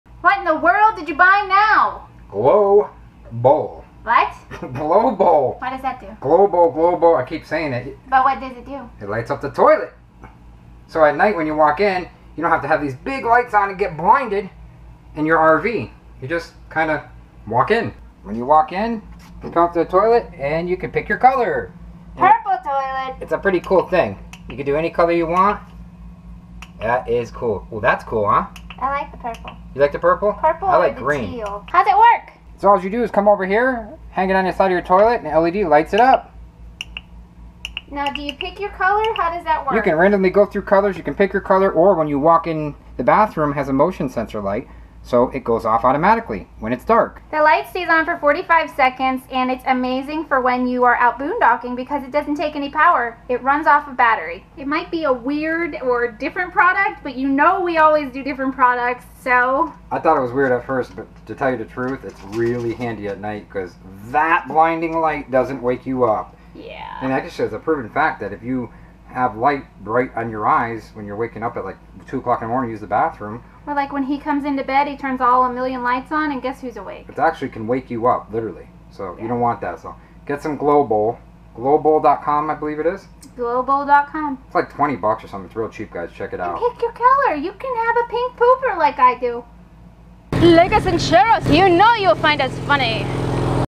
RV Living GlowBowl Led In Your Toilet
GlowBowl is an excellent addition to your RV living arsenal of gadgets. This amazing but simple device turns your toilet into a multi-colored night light. This is a must have for the RVer or Homeowner.